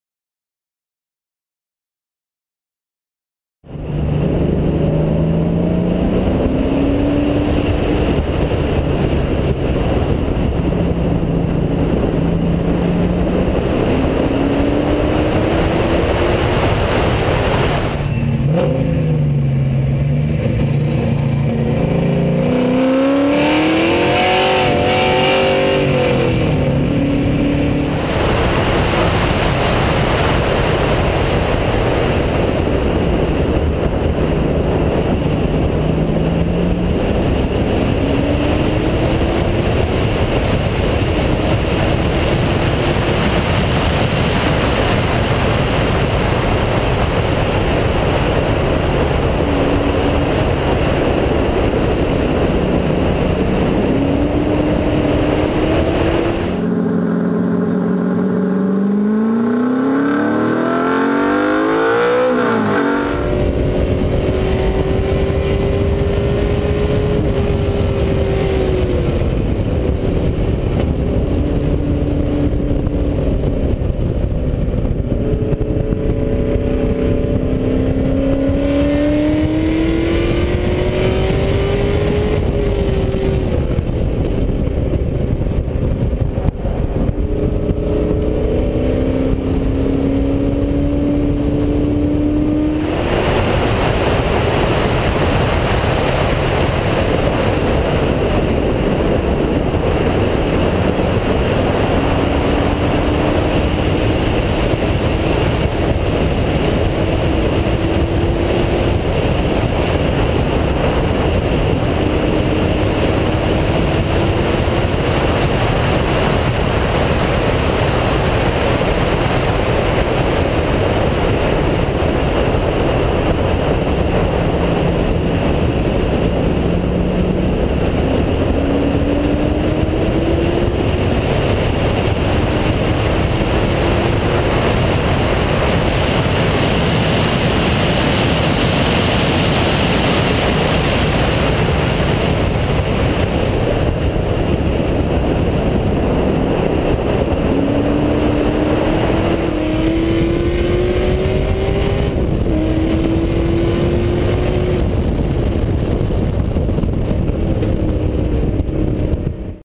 Roadster Shop Chevelle at Gingerman Raceway
Chris Jacobs' 1966 Chevelle equipped with a Roadster Shop Fast Track IRS Chassis takes on Gingerman Raceway.